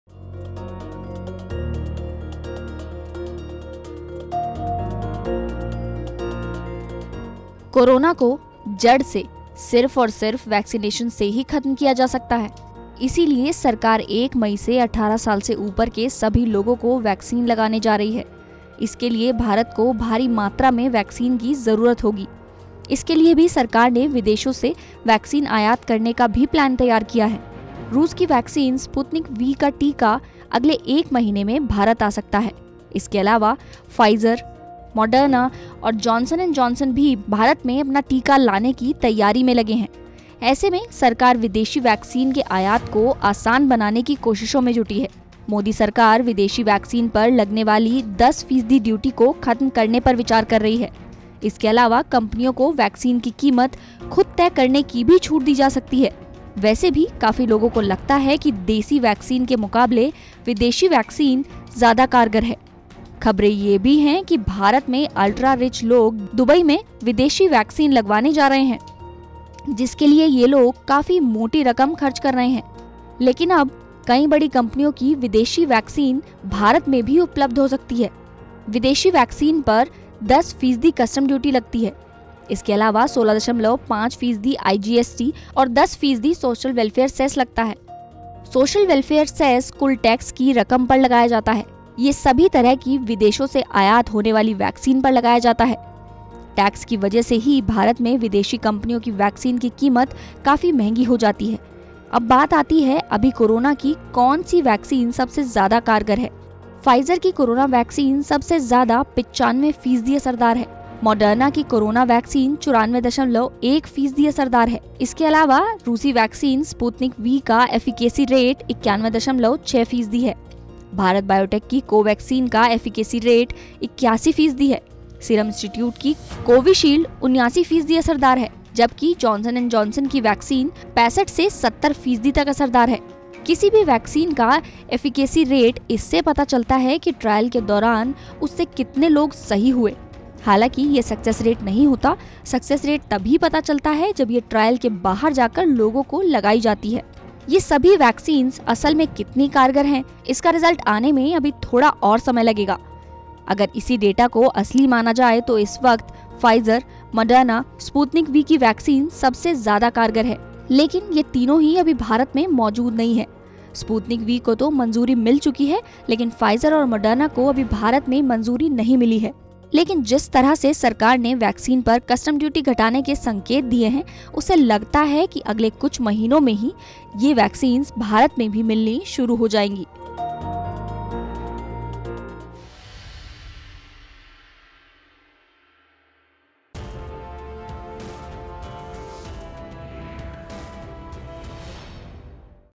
सस्ती होगी विदेशी वैक्सीन! | Biz Tak
कोराना को जड़ से खत्म सिर्फ और सिर्फ वैक्सीनेशन से ही खत्म किया जा सकता है...इसलिए ही सरकार 1 मई से 18 साल से ऊपर के सभी लोगों वैक्सीन लगाने जा रही है...इसके लिए भारत को भारी मात्रा में वैक्सीन की जरूरत होगी...इसके लिए भी सरकार ने विदेशों से वैक्सीन आयात करने का भी प्लान तैयार किया है